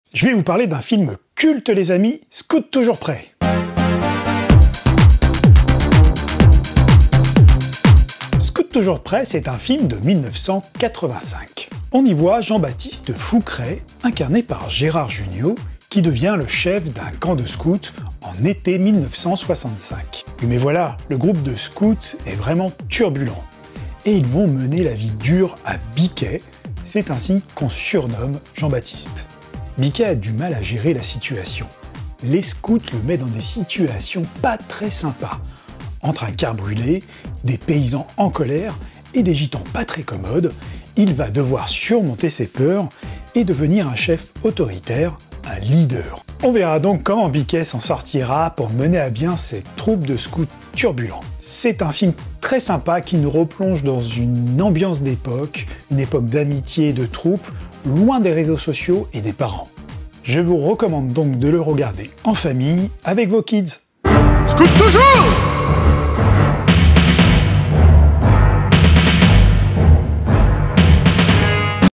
31 mars 2023
Mon film culte aujourd'hui c'est « Scout Toujours »
Avec Gerard Jugnot dans le role d’un chef de clan qui va être malmené par des scouts en pleine forme olympique 😅

a voir en famille avec vos kids 📺